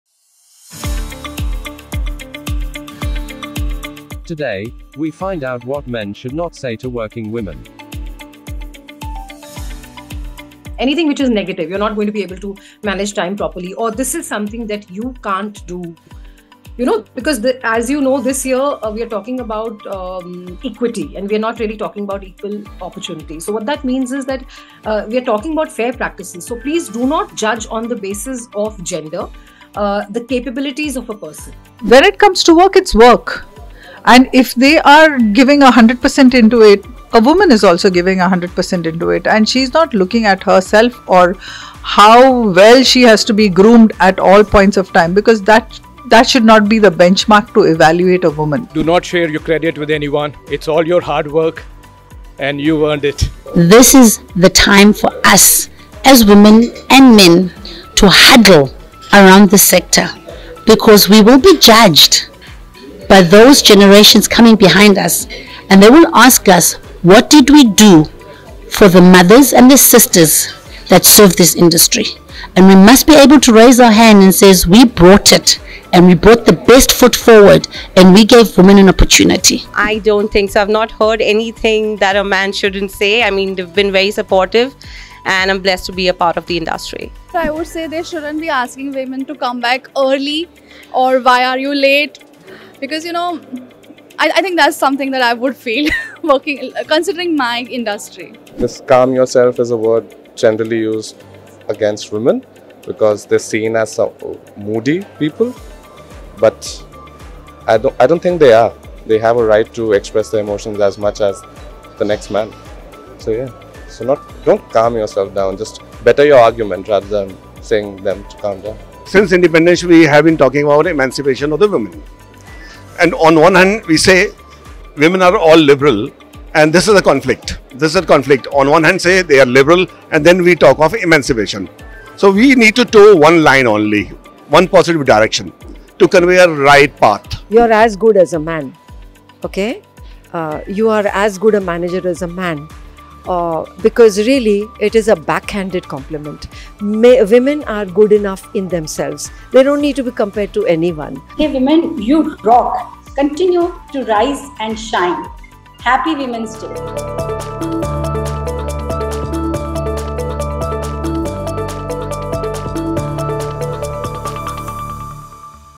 TravelTV.News - Episode 1784 (March 8, 2023)
- In this episode TravelTV.News focuses on:

- Today, we find out what men should not say to working women

- We Interviewed:

- MAMTA PALL, National President- Travel and Tourism Council, Women's Indian Chamber of Commerce & Industry
- SUNIL TALREJA, Vice President – Sales, InterGlobe Air Transport (IGAT)
- KARISHMA KAZI, Owner & Founder, BlackTurtle, India Representative of CORA CORA Maldives Resorts
- NELISWA NKANI, Hub-India, Middle East & South East Asia, South African Tourism
- MADHU SALIANKAR, Executive Director, Global Panorama Showcase (GPS)
- RAJIV NARULA, Founder, The Comforts
- POONAM BHAT, Assistant Vice President-Sales & Training, Travcare Holidays
- RESHMA GODBOLE, Director of Sales- Domestic Leisure, The Leela Palaces, Hotels and Resorts
- ARJUN SINGH, Managing Director, Gloden Blossom, Imperial Resorts
- RANJANA SHARMA, Managing Director, Trav-n-Tours International